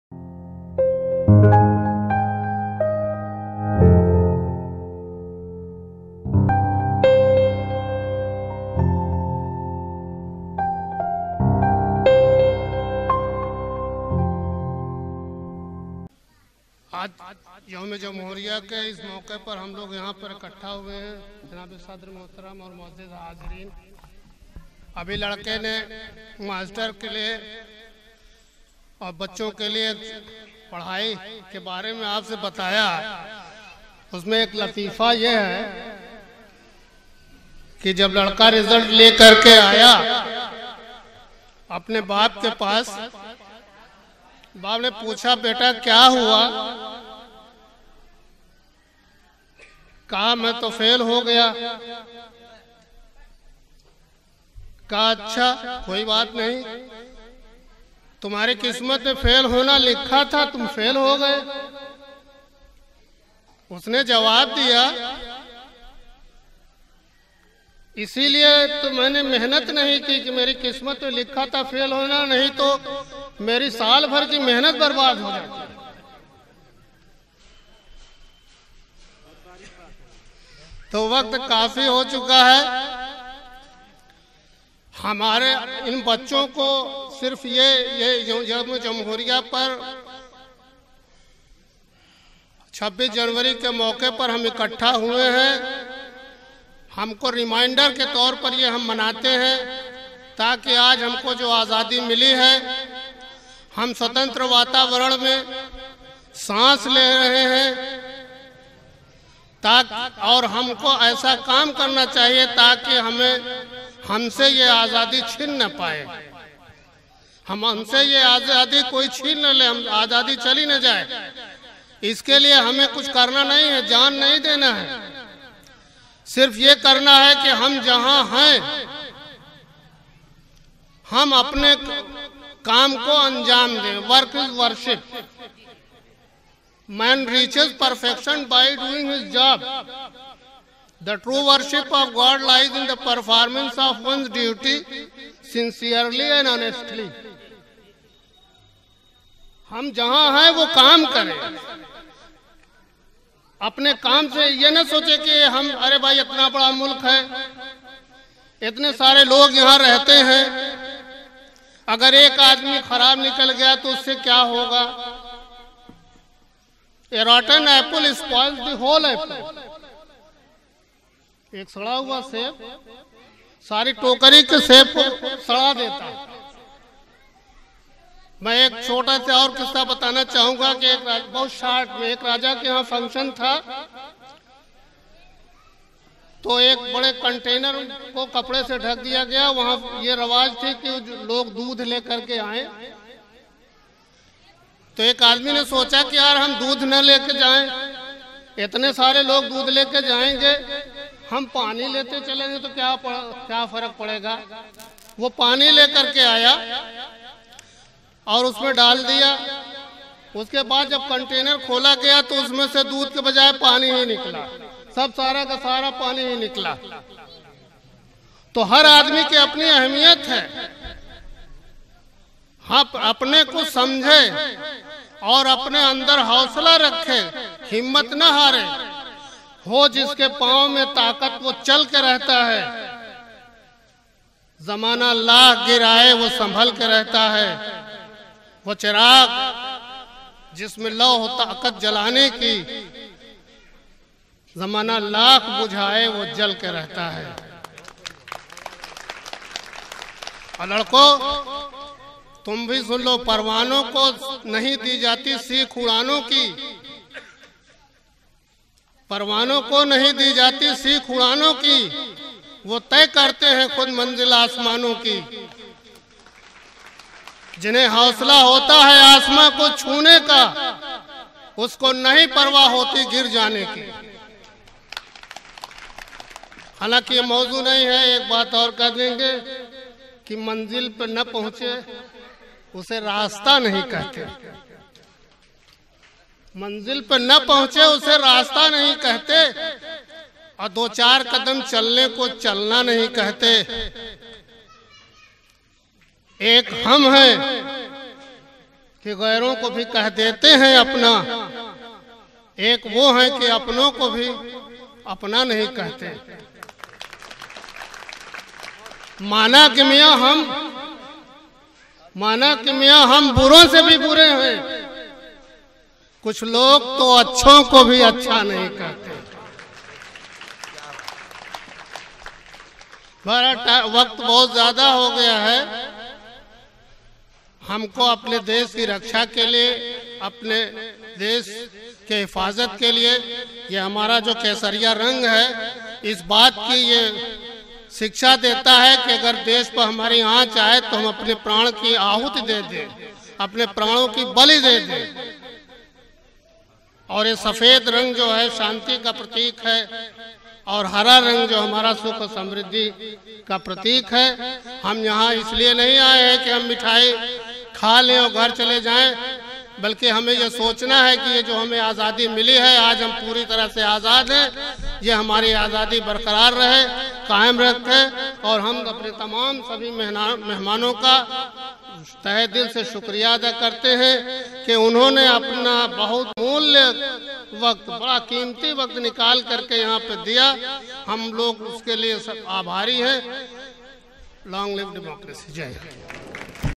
Master Ajaz ( Sarparast Madarsa) in Jashn Yaume Jamhuriya | Madarsa Maulana Abul Kalam Azad
Republic Day Celebration / Gantantra Diwas Samaroh
Madarsa Maulana Abul Kalam Azad
Venue: Darbanipur, Jaunpur U.P.
Dated: 26 january 2019
Chairperson: Mazhar Asif (sadar Markazi Seerat Committe Jaunpur)
Chief Guest: Rehan Hashmi
Guest of Honour: Hasan Wajid Khan
Organiser: Ajwad Qasmi
Govern by : Qasmi foundation Jaunpur
Children Stage Performance
Republic day school Program
School Function
Republic Day Fuction
Students of Madarsa